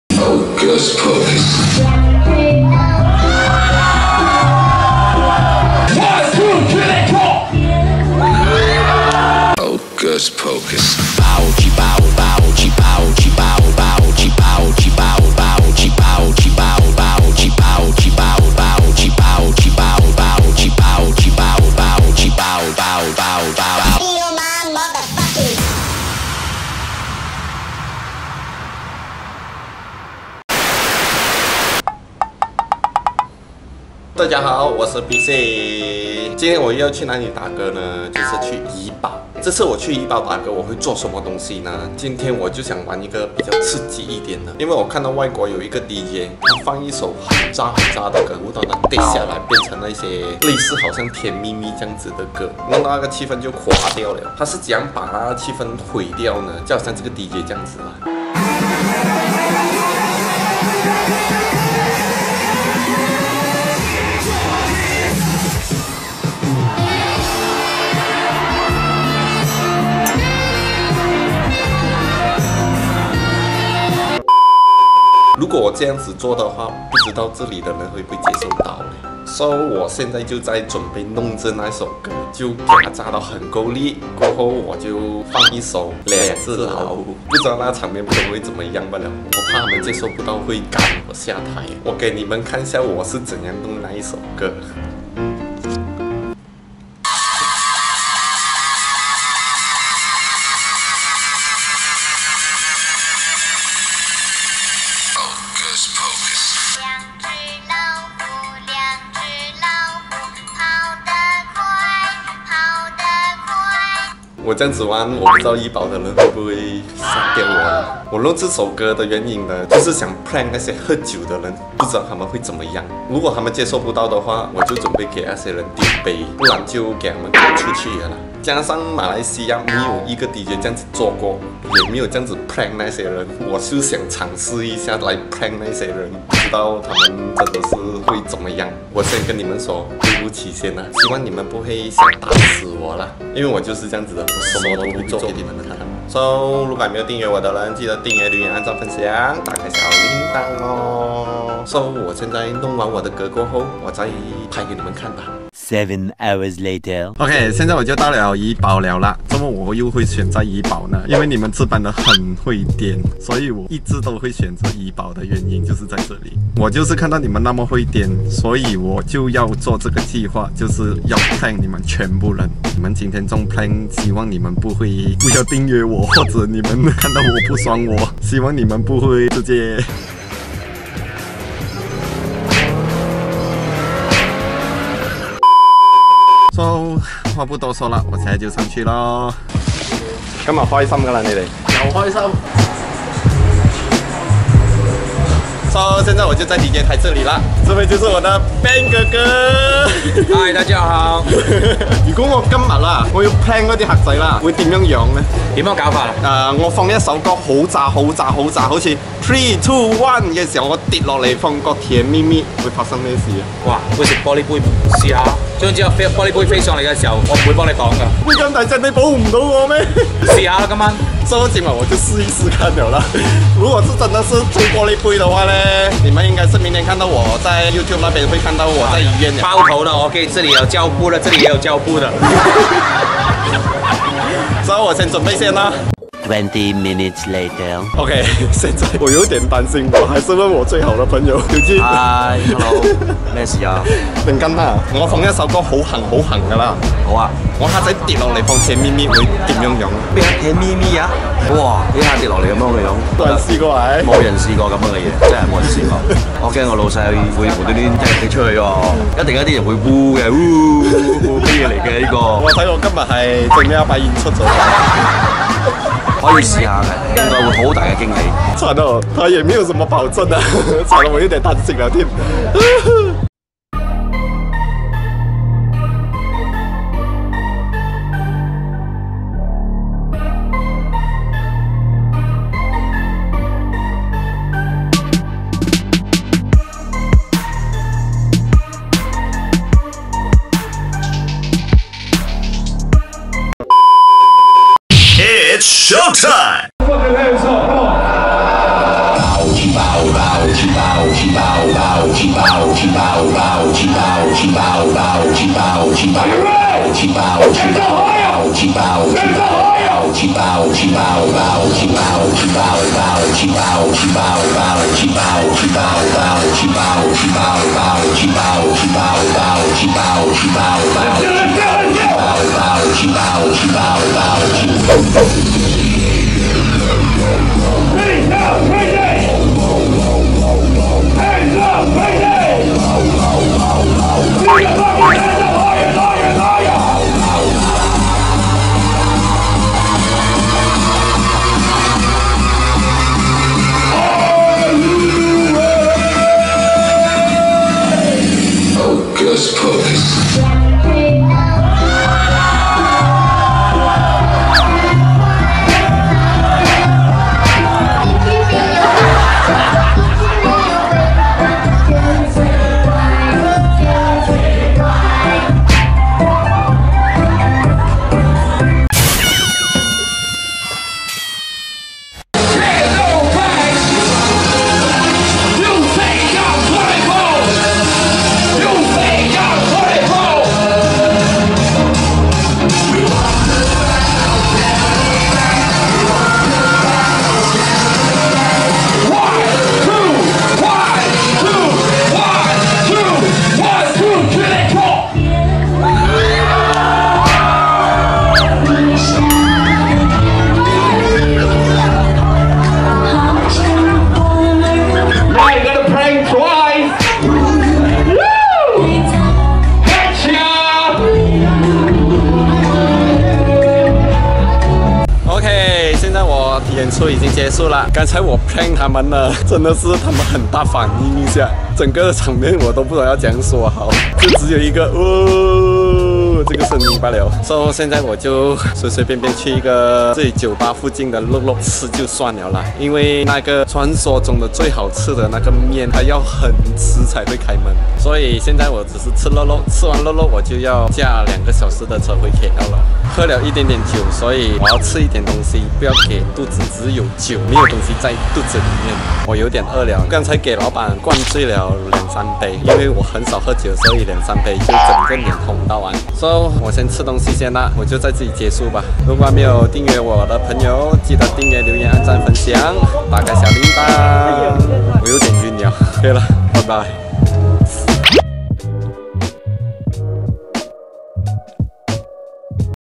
全场人骂DJ乱乱打歌搞毁所有气氛 Prank恶整 18禁 (BeeC#8)
今后我会拍摄更多别的搞笑和DJ有内涵有内容的影片！
真心的谢谢你们的支持！

...└┐▒▒▒▒┌┘and...